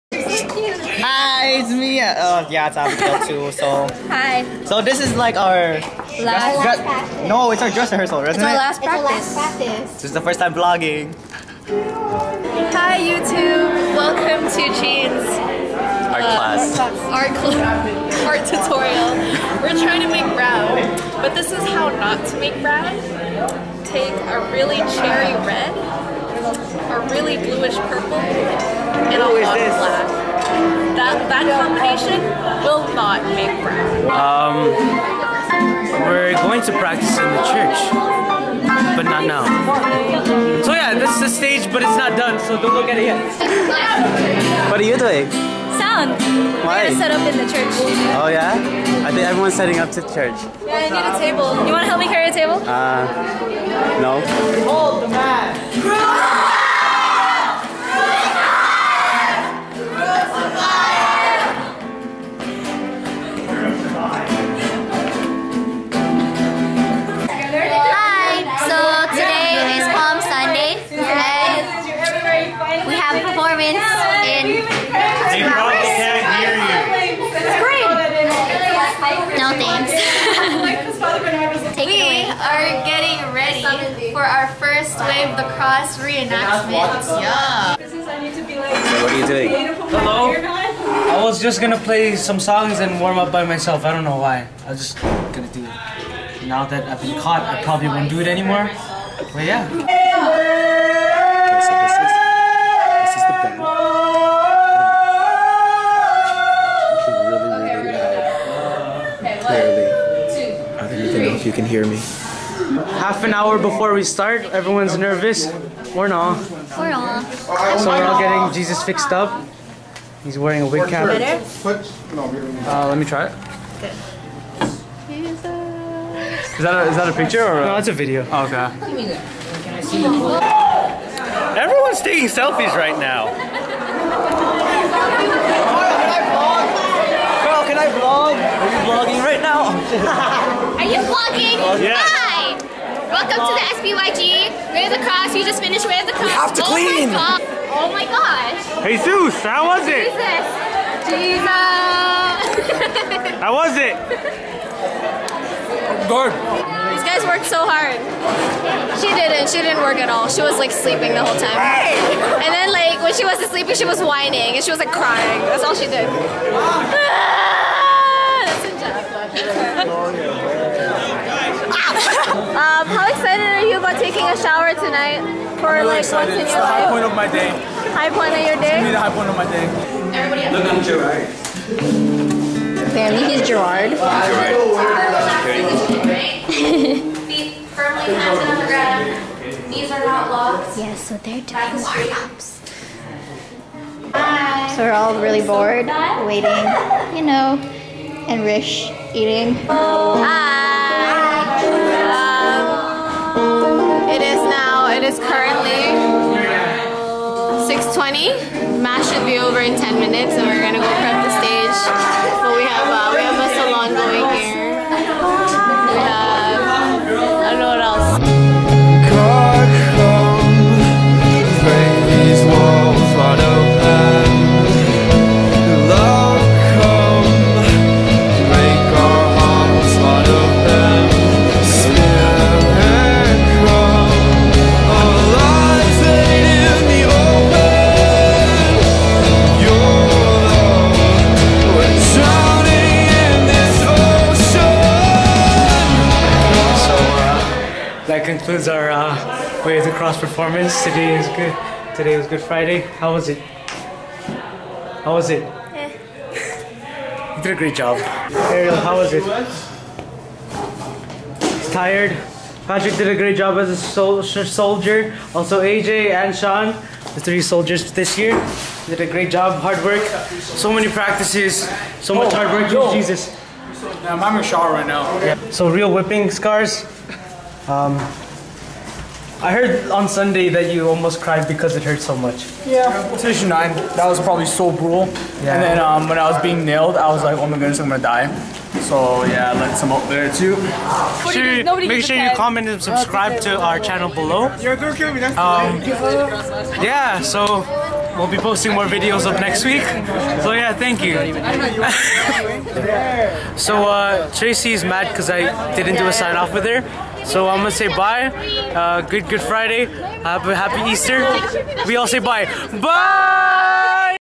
WOTC 2014 - Vlog 4
Our final Vlog for the 2014 Way of the Cross. Special thanks to everyone who helped make this year's Way of the Cross a success!

JEEEEESSSSSSSUUUUUUUUSSSSSSSSSS